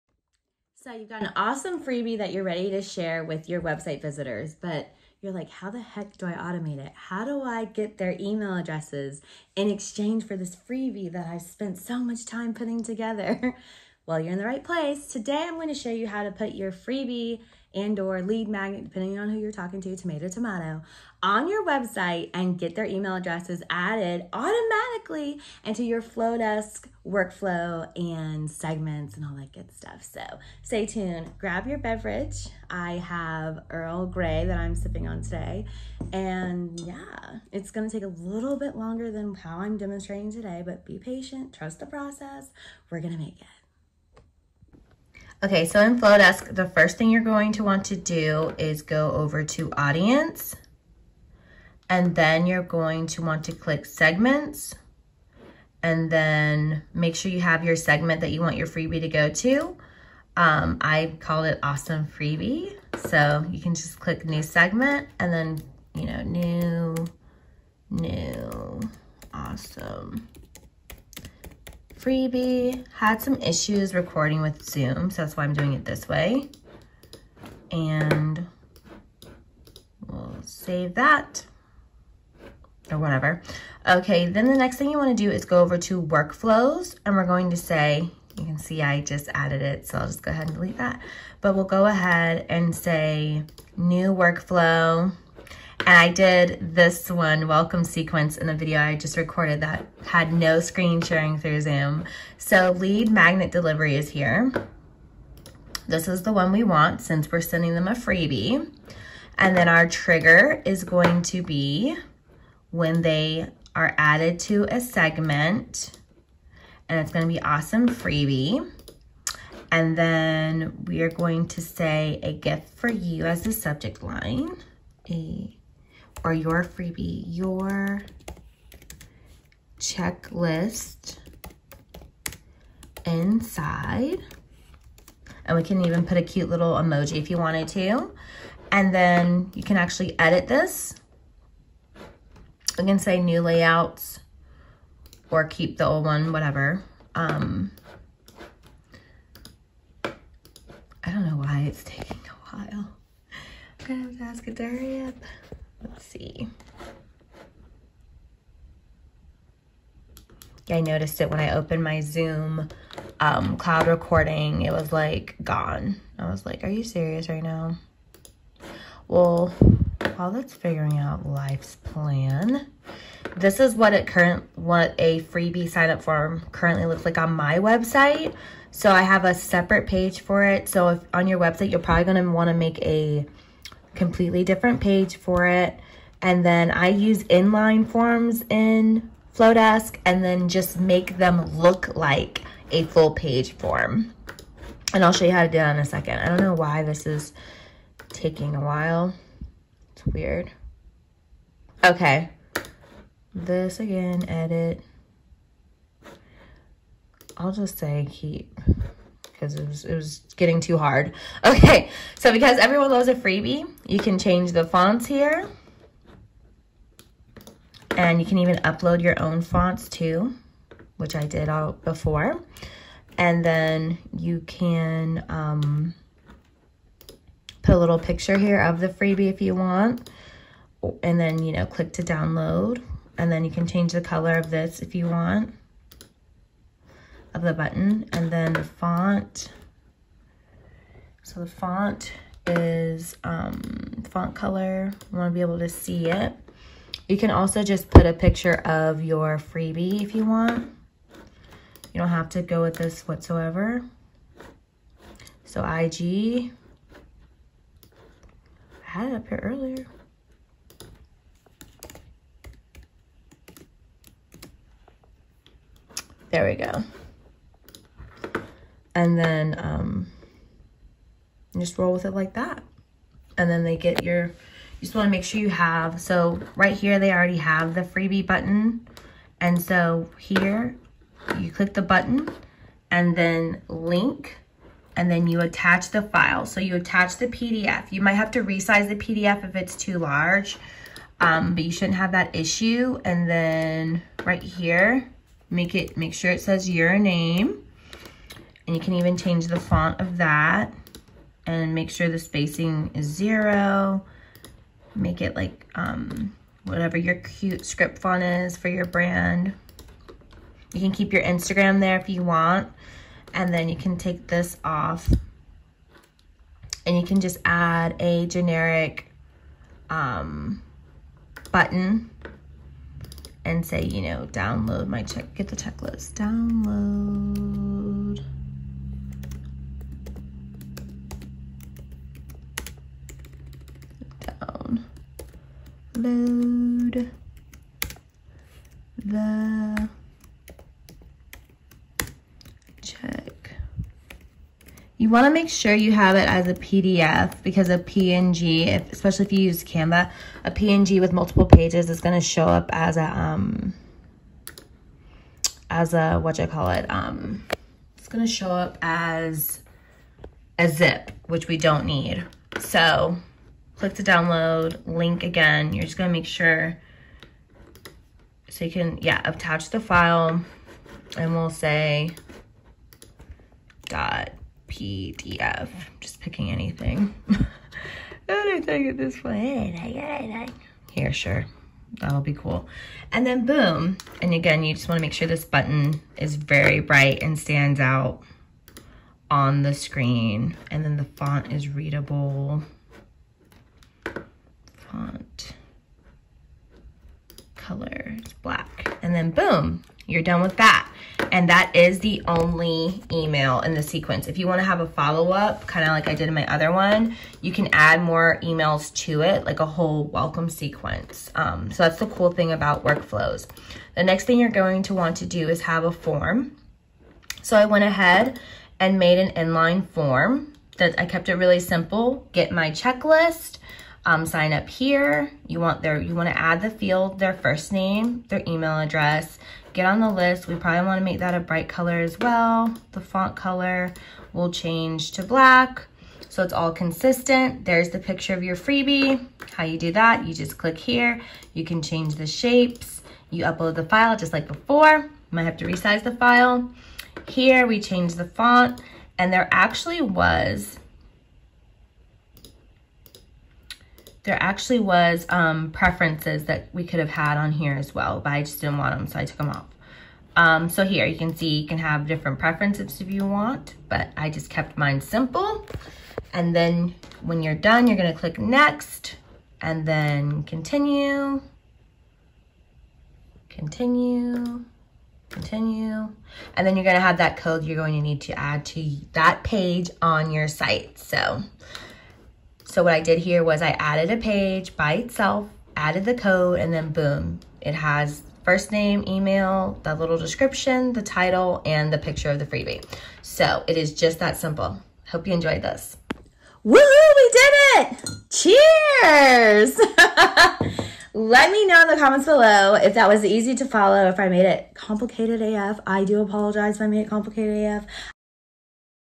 HOW TO ADD freebies TO YOUR WEBSITE WITH FLODESK
Ready to share your awesome freebie with the world, but not sure how to add it to your website?  Today, we're going to add a freebie to our website with Flodesk!

Mentioned in this video:
- Audience
- Segment
- Workflow
- Sign Up Form
- HTML code

Try Flodesk!